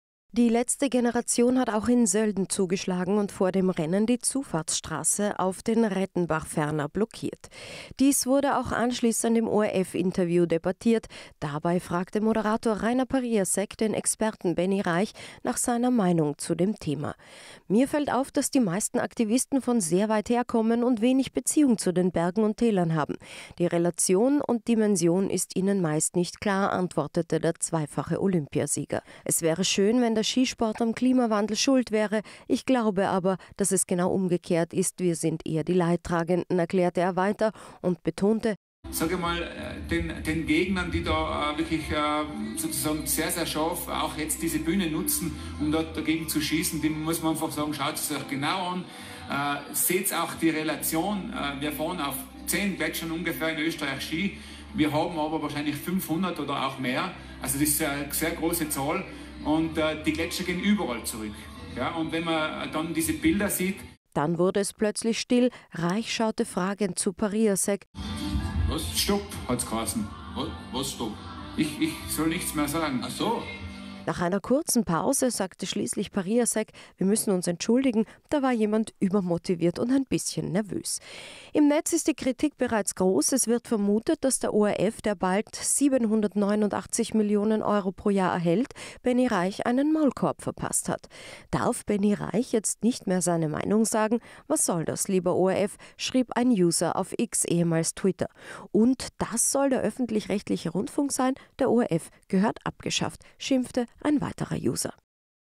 "Darf nichts mehr sagen": ORF unterbricht Benni Raich während Klima-Debatte
Die “Letzte Generation” hat vor dem Ski-Rennen in Sölden die Zufahrtsstraße blockiert. Für größeren Wirbel sorgt jedoch aktuell ein anschließendes ORF-Interview mit Benjamin Raich. Dem Doppel-Olympiasieger wurde bei der Klima-Debatte offenbar ein Maulkorb verpasst.